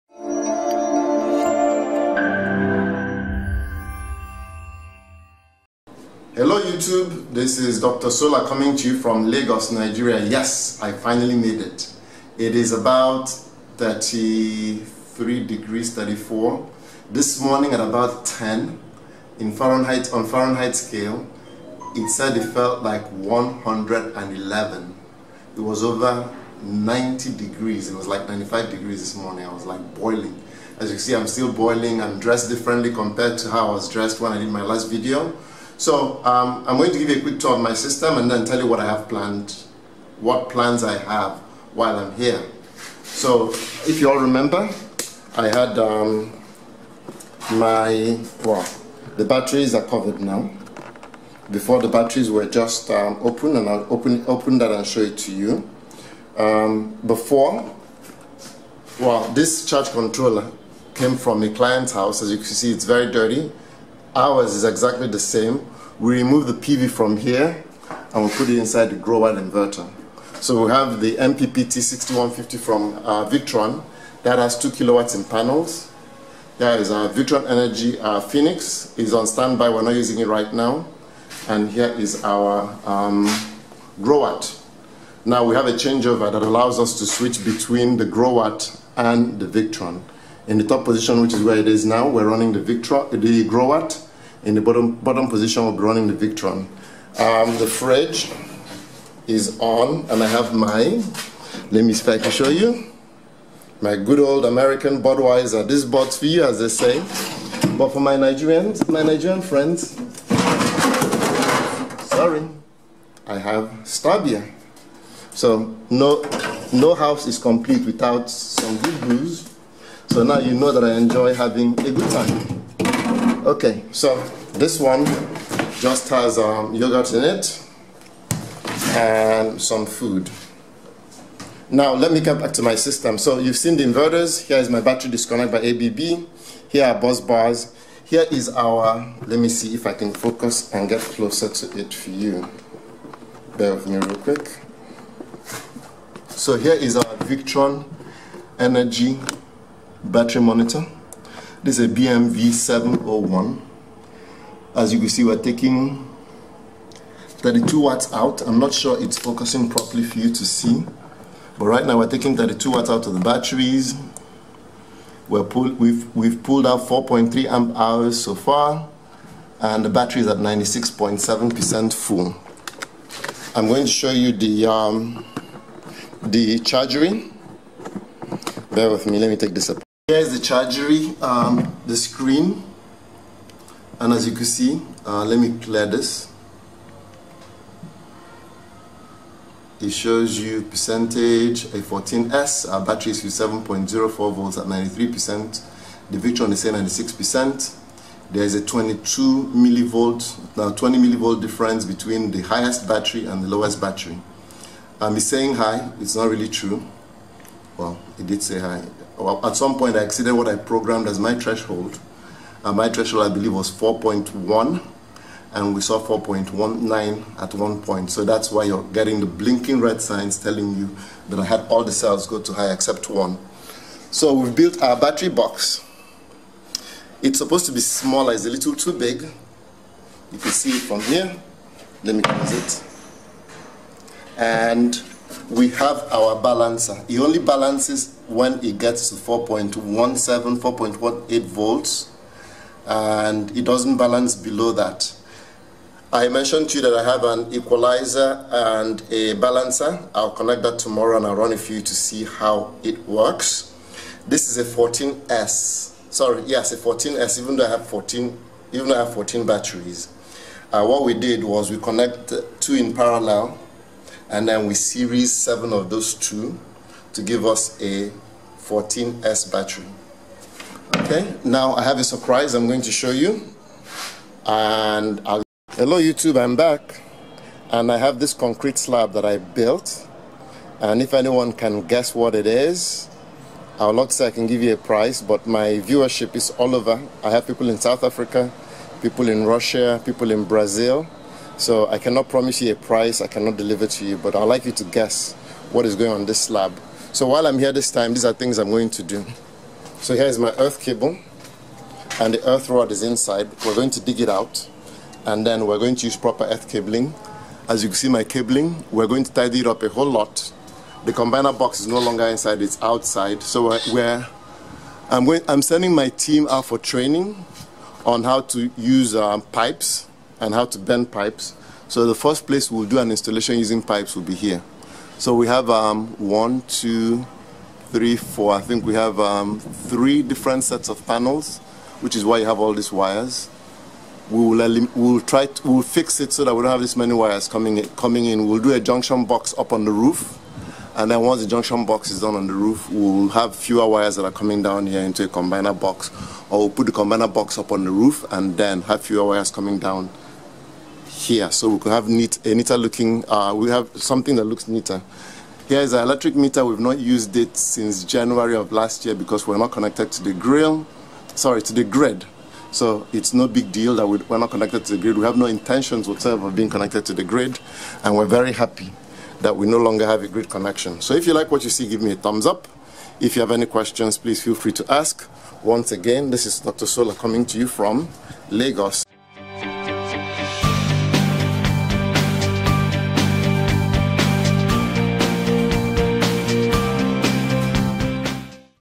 My off grid set up plus beer in the fridge.
Quick tour of my 100% off grid system, balancers, Beer in the fridge and more.
Schneider Conext MPPT 60 150 Charge Controller
Victron Energy 60 150 MPPT Charge Controller
Victron Energy 3 kva Phoenix Inverter
Growatt New Energy SPF5000 Inverter
Victron BMV Battery monitor